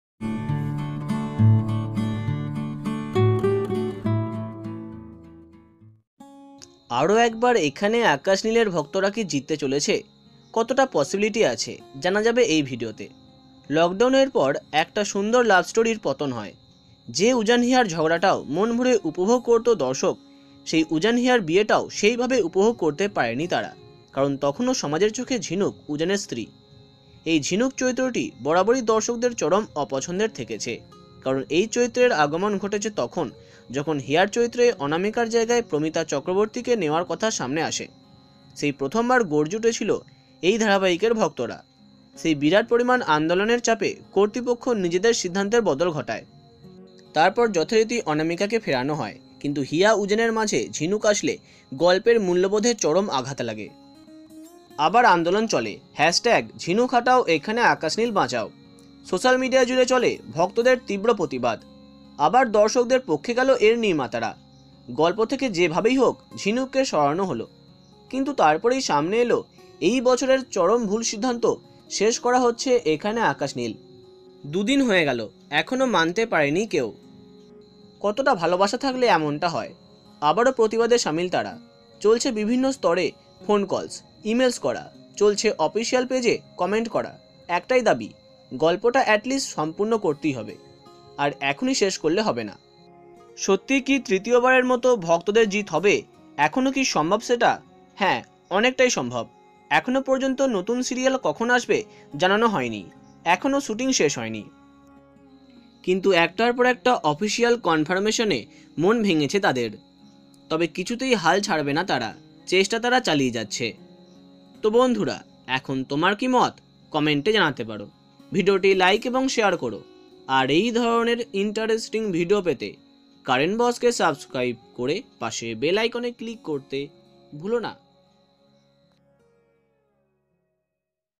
এখনও ফেরানো যাবে এখানে আকাশ নীল ?কতটা সম্ভব জানুন |Ekhane akash neel|Star jalsha
Ekhane akash neel |Star jalsha |Sean banerjee|Anamika Chakraborty

Artist-Aaron Kenny